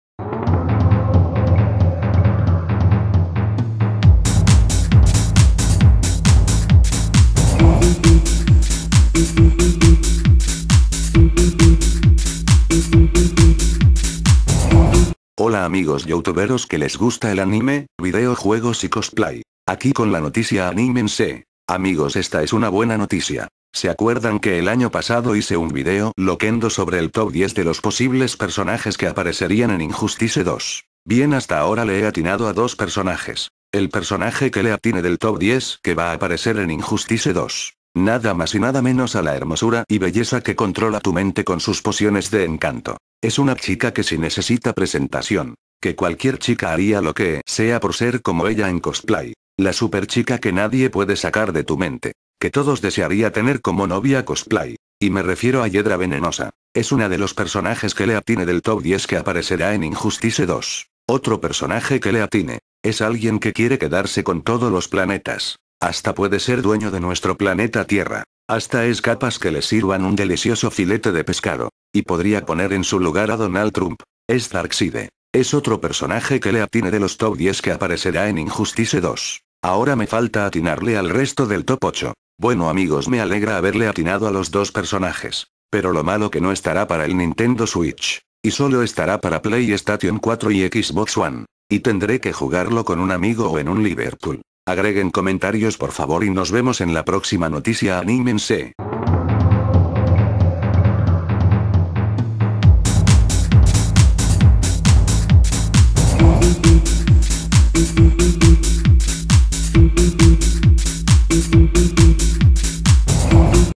LA NOTICIA ANIME-NSE PRESENTA ---- los 2 personajes que le atine que apareceran en injustice 2
agreguen comentarios porfavor sobre los 2 personajes que le atine